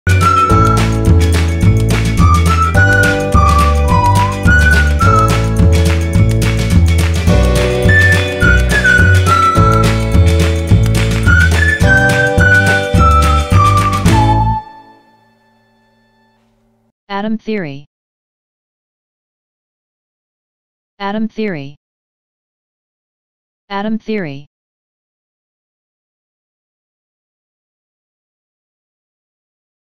Atom theory Nedir? Atom theory İngilizce Türkçe Anlamı Ne Demek? Nasıl Okunur? Çeviri Sözlük
Atom theory Türkçe anlamının ne olduğunu öğrenin. Atom theory nedir öğrenin. Atom theory İngilizce cümle içerisinde kullanın ve İngilizce olarak okunuşunu öğrenin. Atom theory videosu ile online çevirisini izleyin.

Atom theory hakkında daha fazla bilgi almak, örnek cümle içerisinde kullanımını görmek için: